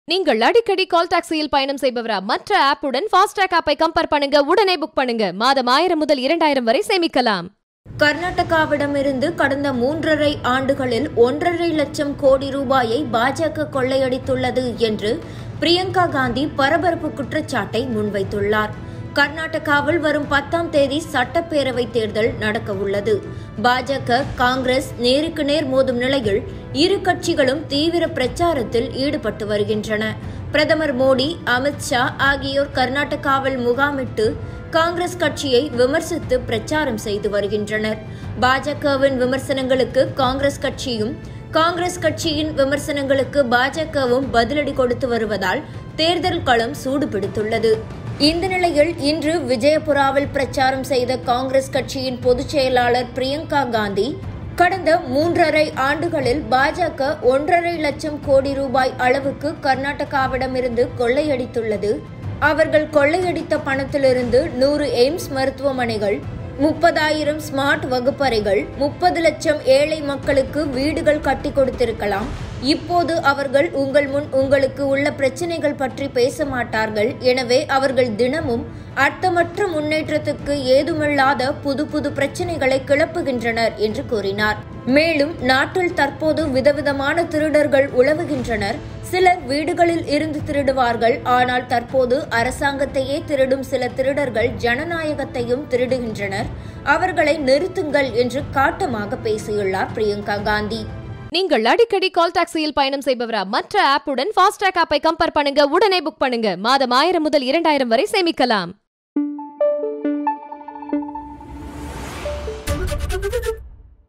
Priyanka Gandhi | "கர்நாடகாவில் பாஜக ரூ.1.5 லட்சம் கோடி கொள்ளையடித்துள்ளது" பிரியங்காகாந்தி பகீர்..
Chennai first call taxi since 2001 fasttrack Cabs